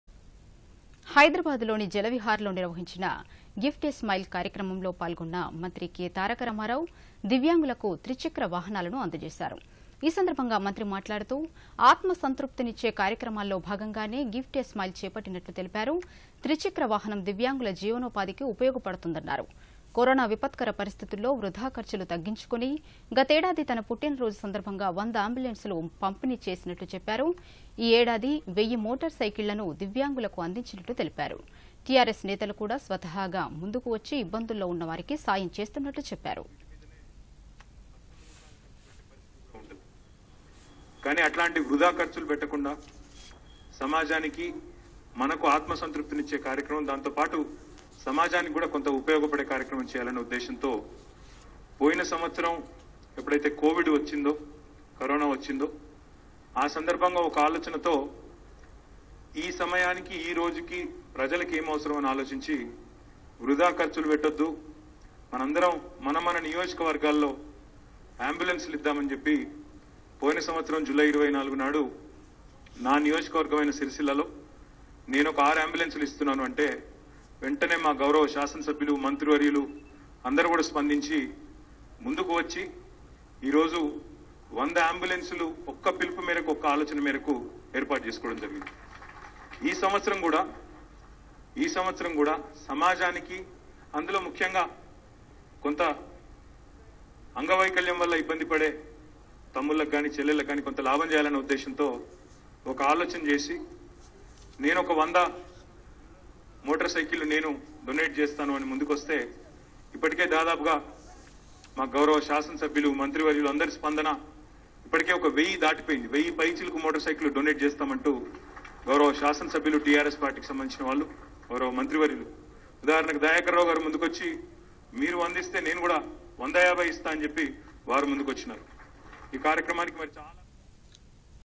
KTR :Tricycles were donated to the paraplegics under Gift A Smile
హైదరాబాద్ లోని జలవిహార్ లో నిర్వహించిన గిఫ్ట్‌ ఏ స్మైల్‌ కార్యక్రమంలో పాల్గొన్న మంత్రి కె.తారకరామారావు... దివ్యాంగులకు త్రిచక్ర వాహనాలను అందజేశారు. ఈ సందర్భంగా మంత్రి మాట్లాడుతూ... ఆత్మ సంతృప్తినిచ్చే కార్యక్రమాల్లో భాగంగానే గిఫ్ట్‌ ఏ స్మైల్‌ చేపట్టినట్లు తెలిపారు. త్రిచక్ర వాహనం దివ్యాంగుల జీవనోపాధికి ఉపయోగపడుతుందన్నారు. కొరోనా విపత్కర పరిస్థితుల్లో వృథా ఖర్చులు తగ్గించుకుని గతేడాది తన పుట్టిన రోజు సందర్భంగా 100 అంబులెన్స్‌లు పంపిణీ చేసినట్లు చెప్పారు. ఈ ఏడాది వెయ్యి మోటార్‌ సైకిళ్లను దివ్యాంగులకు అందించినట్లు తెలిపారు. టీఆర్‌ఎస్‌ నేతలు కూడా స్వతహాగా ముందుకు వచ్చి ఇబ్బందుల్లో ఉన్నవారికి సాయం చేస్తున్నట్లు చెప్పారు.